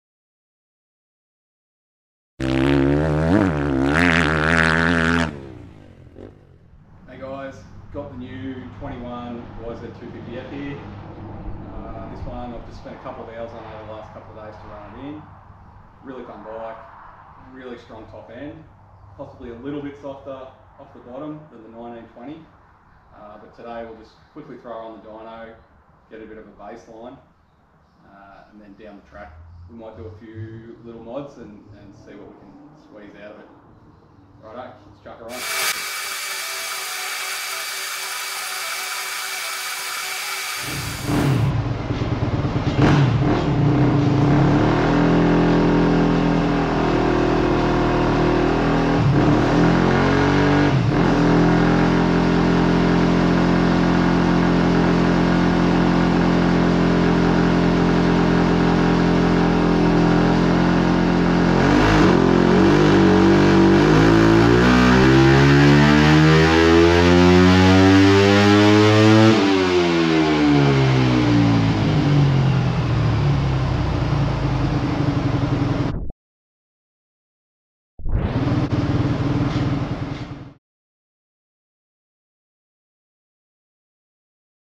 MOTO TRUTH: 2021 YZ250F DYNO TEST
Quick shake down run on the 2021 Yamaha YZ250F. More mods and videos to come on this one.